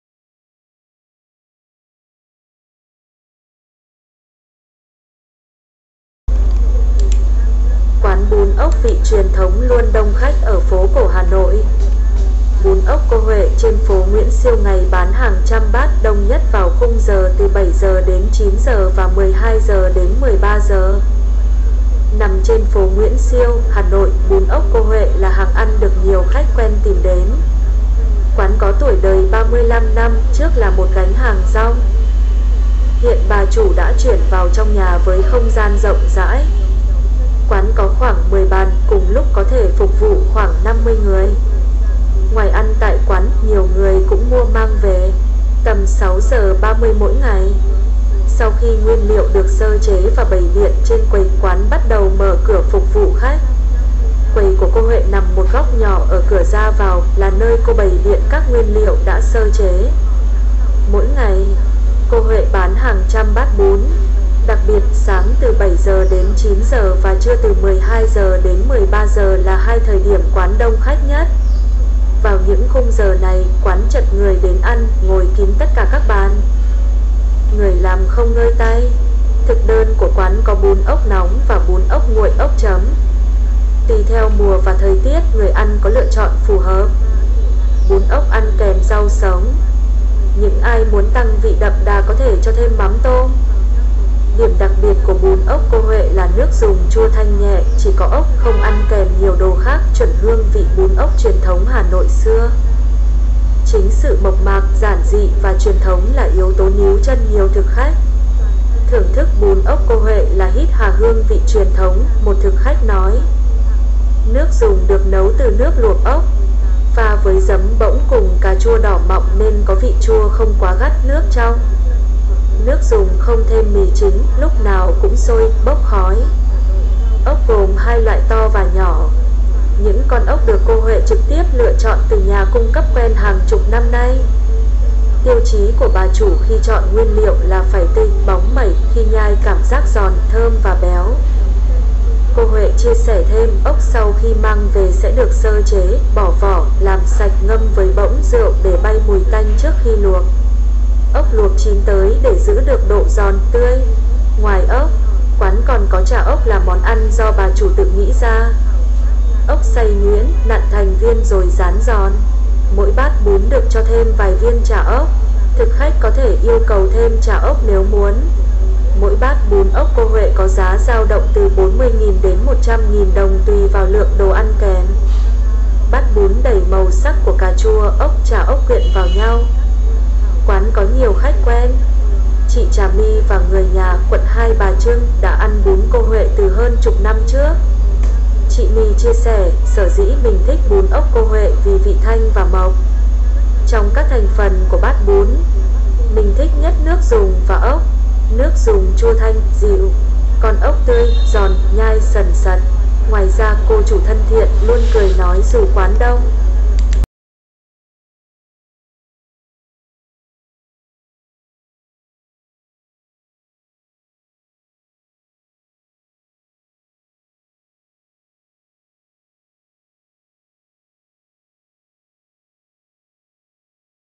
Hàng bún ốc chuẩn vị phố cổ Hà Nội-Snail vermicelli in the old town Hanoi
Quán bún ốc vị truyền thống luôn đông khách ở phố cổ Hà NộI. Bún ốc cô Huệ trên phố Nguyễn Siêu ngày bán hàng trăm bát, đông nhất vào khung giờ từ 7h đến 9h và 12h đến 13h.
The traditional snail noodle shop is always crowded in Hanoi's old quarter. Ms. Hue snail vermicelli on Nguyen Sieu street sells hundreds of bowls a day, the most crowded in the time frame from 7am to 9am and 12pm to 1pm.